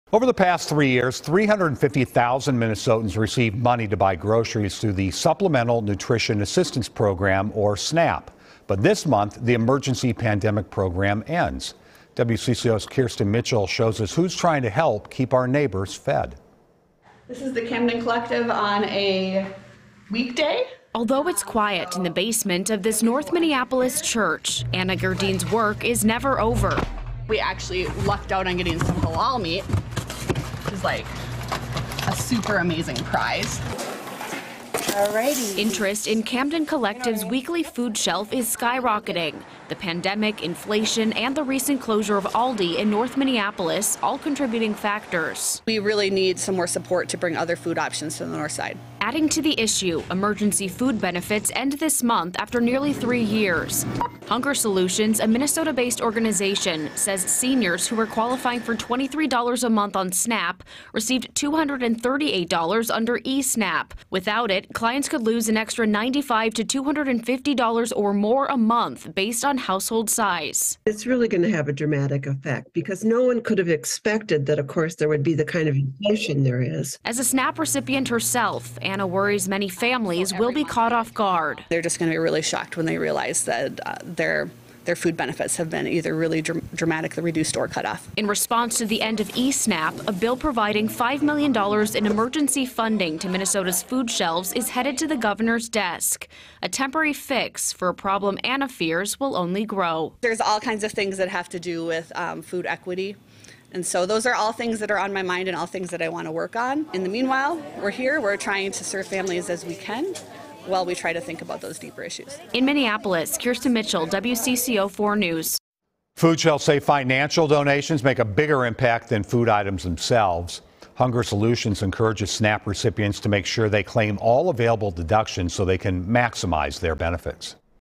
Food shelves brace for increased demand after end of E-SNAP
Hunger Solutions, a Minnesota-based non-profit says for example, seniors who were qualifying for $23 dollars on SNAP, received $238 dollars under E-SNAP. Without it, clients could lose an extra $95 to $250 or more a month based on household size.